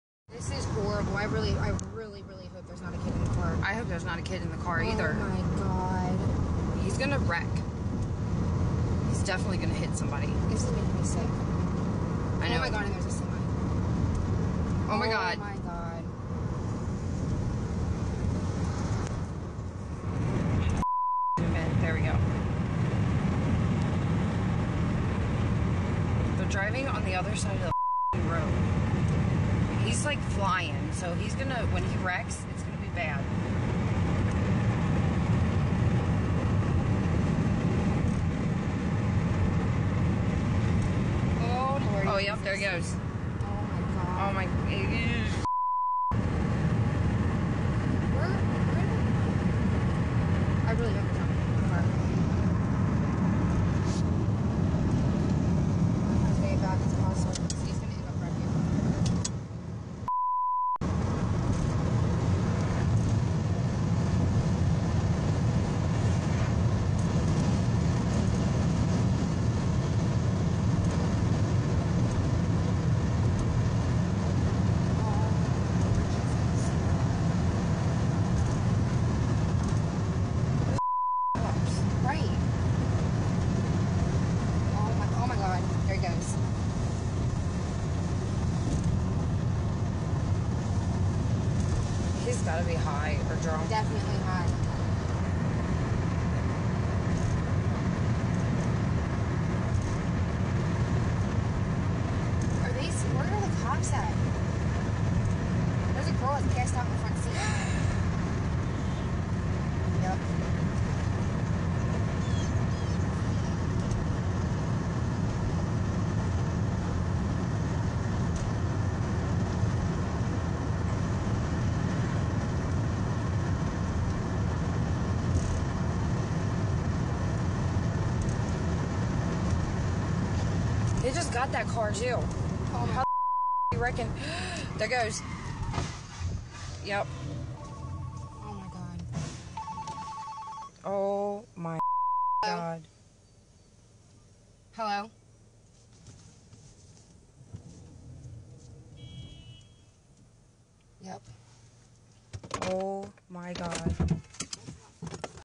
RAW VIDEO: Woman captures intoxicated driver on video before head-on crash
A woman and her passenger captured an erratic driver on State Route 128 near Harrison before the driver hits a truck head-on.